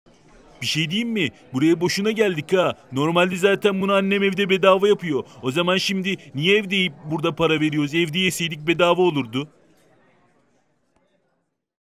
Bunu evde bedava yerdik! -maladam- Animasyon
''maladam'' Sorulamayanları soruyor, sizi çileden çıkarıyor! o her yerde, durakta, otobüste ayakta,sokakta boş bir bankta, kameraların olduğu her anda ! ''maladam'' karşınızda!

Animasyon ,Yazar,Çizer,Müzik,Seslendirme : Tarkan Ayaş

Not : İzlediğiniz içerikteki, kişiler, mekanlar ve karakterler tamamen kurgu ve hayal ürünüdür, kimse ile bir bağlantısı yoktur. Benzerlikler var ise tesadüfidir.

maladam bir ''Gariplik Animasyon'' eseridir.

Tarkan Ayaş